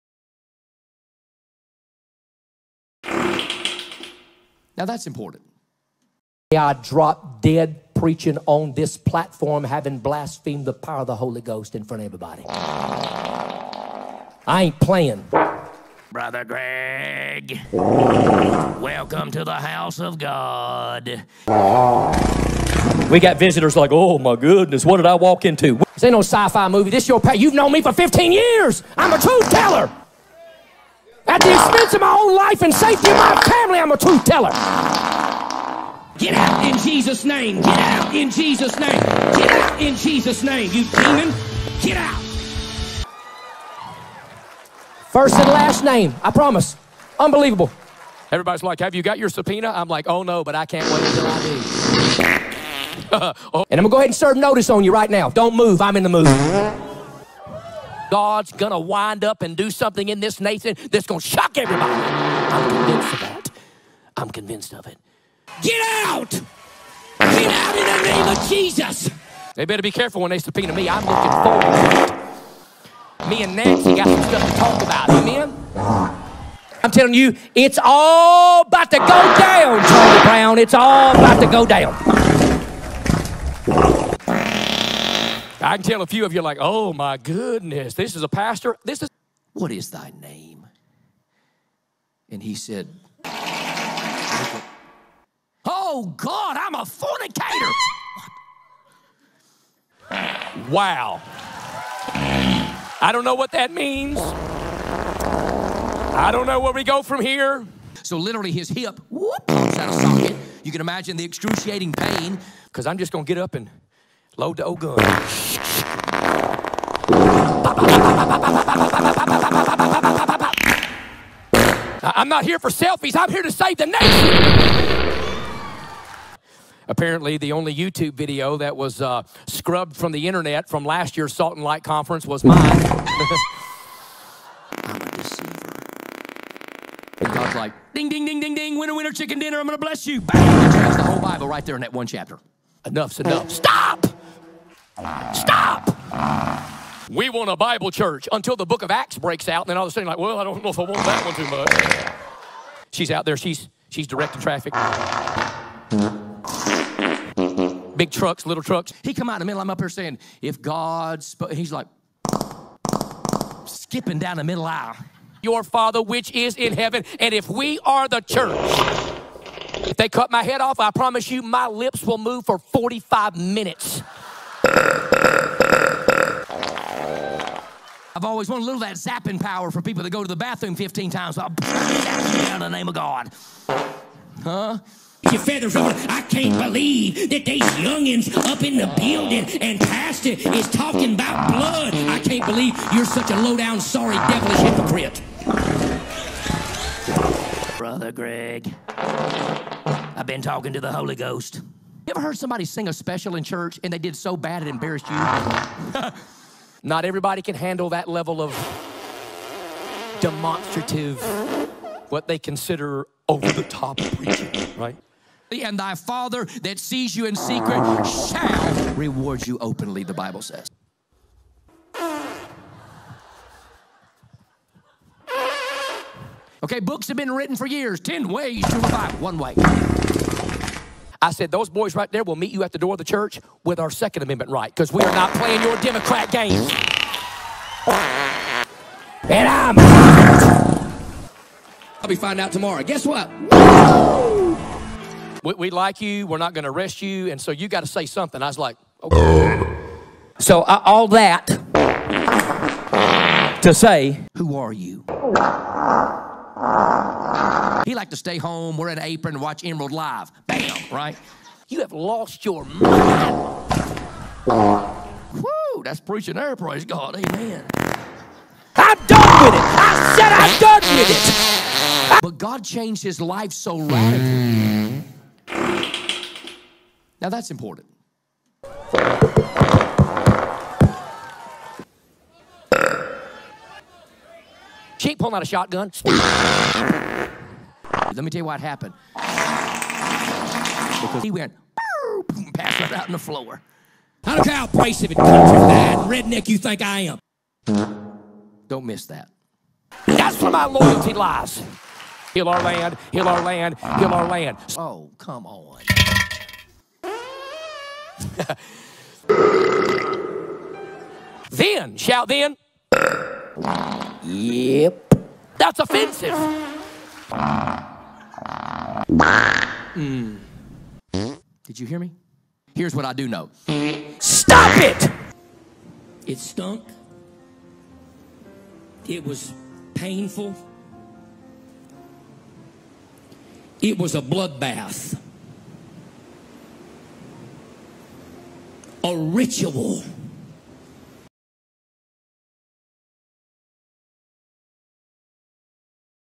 FARTING PREACHER 2023 - Brother Greg Locke & The Demonic Colonic
Brother Greg Locke exorcises intestinal demons.